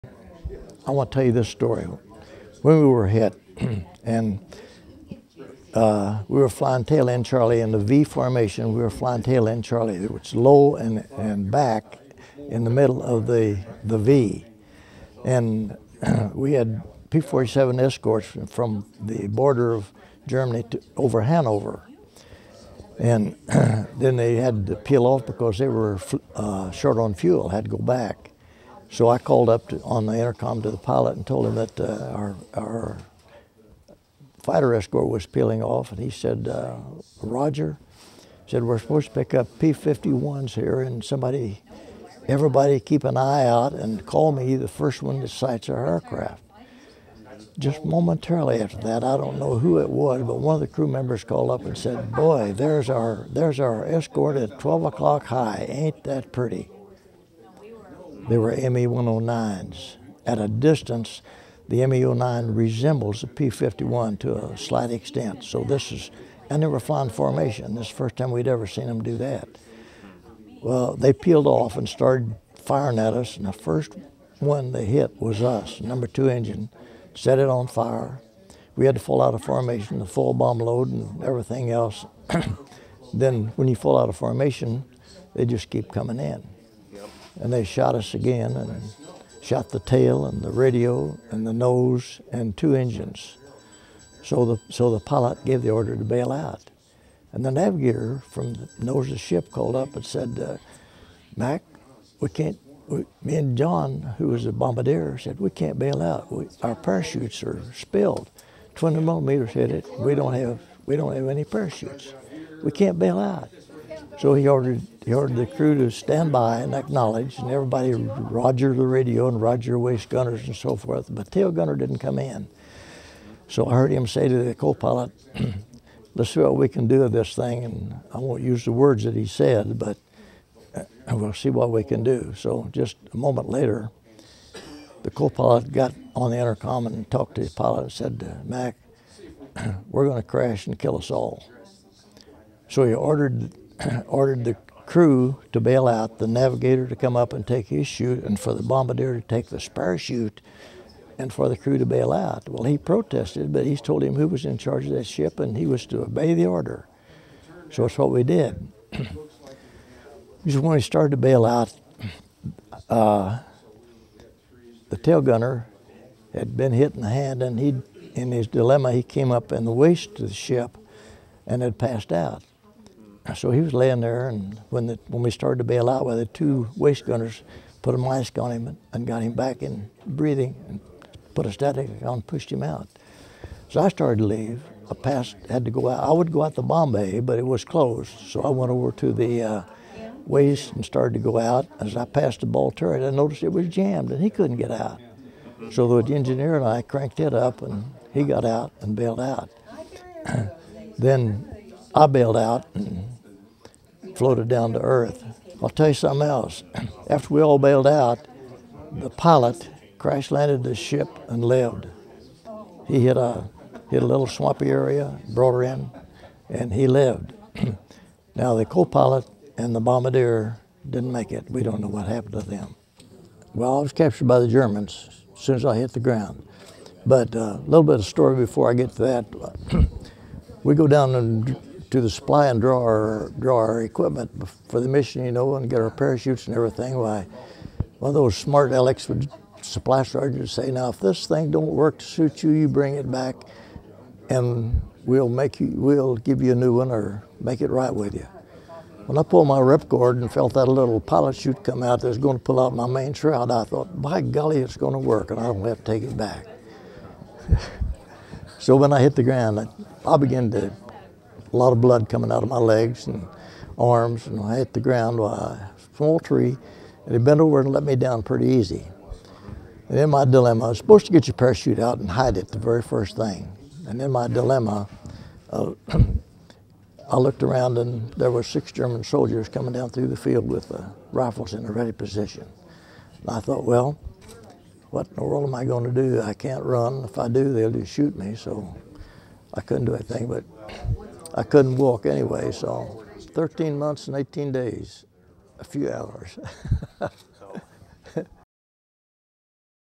WWII veteran describes being shot down, becoming POW
90 year-old Wilburn Rowden is a distinguished veteran of World War II, having served in the Air Force. On March 8, 1944, his plane was shot down by German forces. When he and the crew of his B-17 bailed out, he was immediately captured by German soldiers. Rowden was a prisoner of war in Germany for 13 months and 18 days.